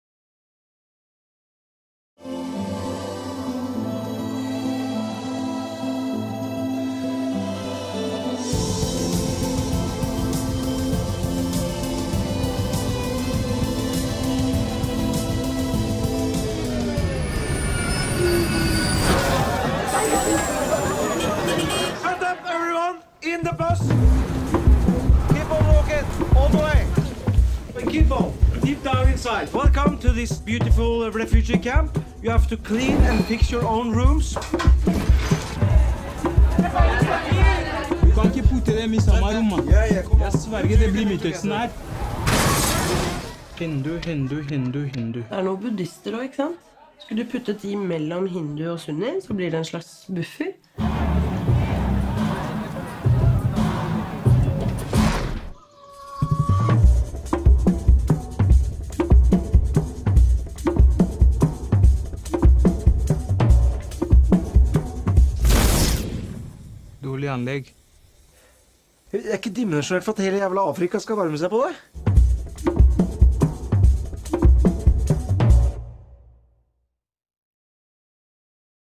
Zurich Film Festival 2016 | arttv Tipp: Welcome to Norway
Publikumspreise sind zwar nicht die renommiertesten eines Festivals, sagen aber am meisten darüber aus, ob ein Film gefällt. «Welcome to Norway» hat zwei davon und ist einer der Filme des Zurich Film Festivals, die man nun wirklich nicht verpassen sollte.